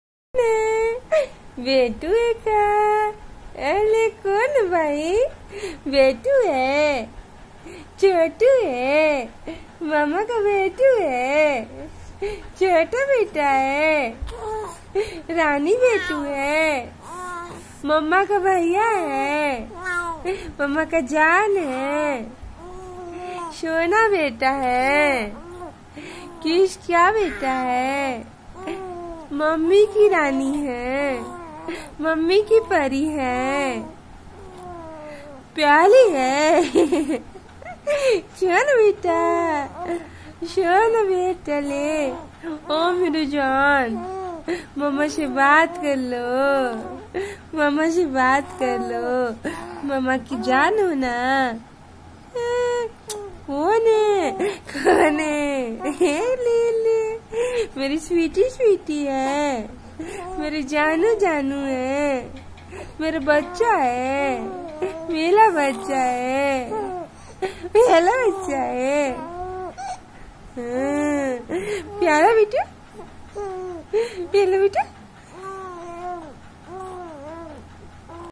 Cute baby enjoying with mumma
3months old baby talking with his mother.